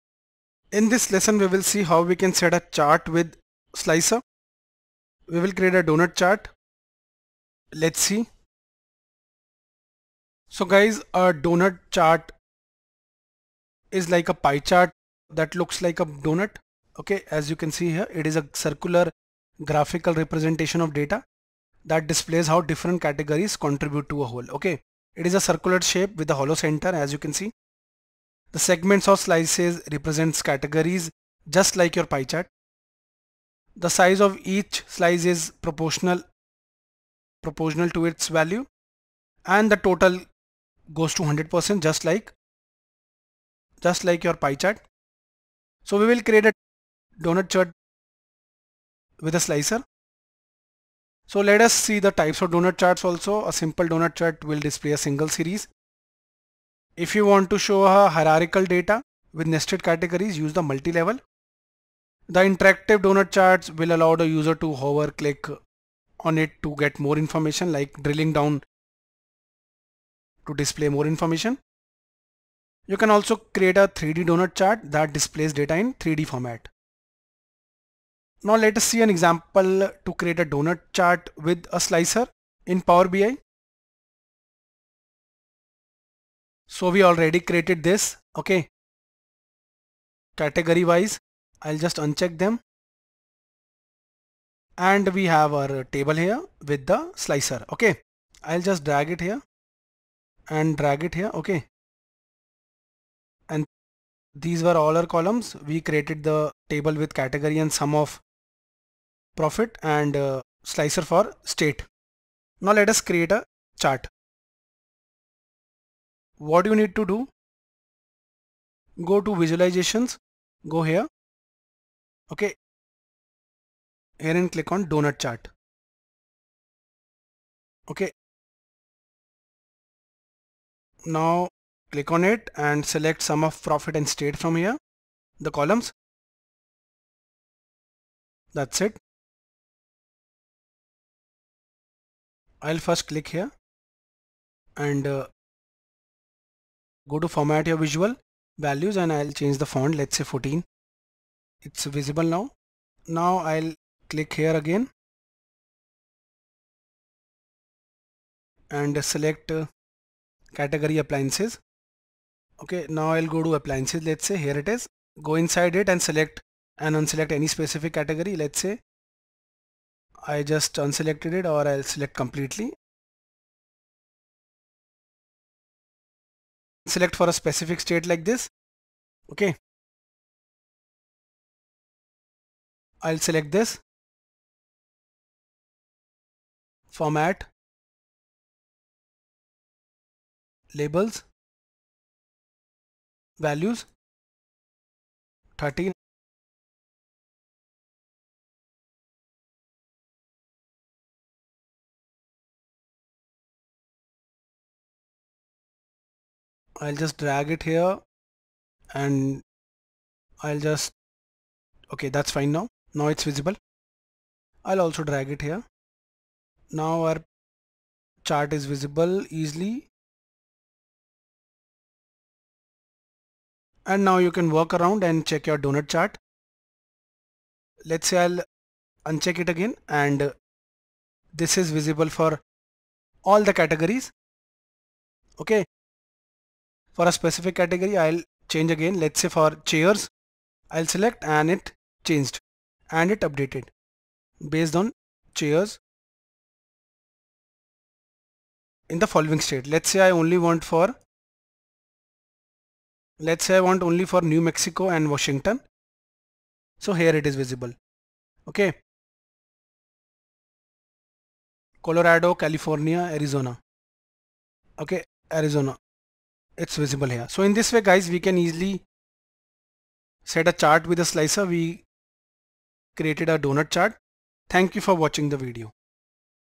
How to set a chart with a slicer in Power BI | Power BI Tutorial | Amit Thinks | 2025
Let us set up a Donut Chart with a slicer in Power BI. Slicers are Interactive Filters. Consider it an interactive visual filter to narrow down the data displayed in reports.